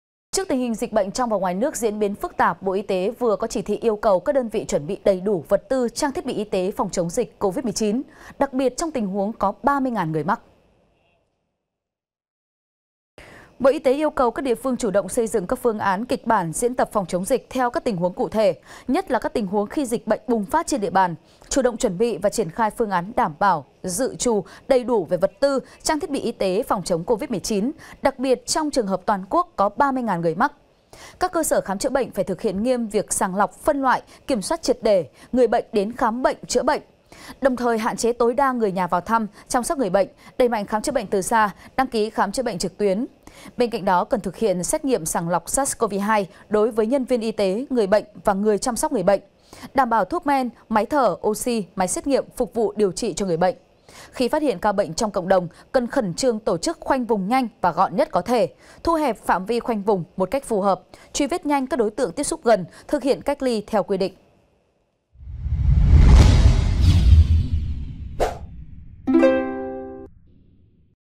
Bộ Y tế chuẩn bị kế hoạch khi có 30.000 ca mắc Covid-19 | VTC14
VTC14 | BỘ Y TẾ CHUẨN BỊ KẾ HOẠCH KHI CÓ 30.000 CA MẮC COVID-19
Trước tình hình dịch bệnh trong và ngoài nước diễn biến phức tạp, Bộ Y tế vừa có chỉ thị yêu cầu các đơn vị chuẩn bị đầy đủ vật tư, trang thiết bị y tế phòng, chống dịch Covid-19, đặc biệt trong tình huống có 30.000 người mắc.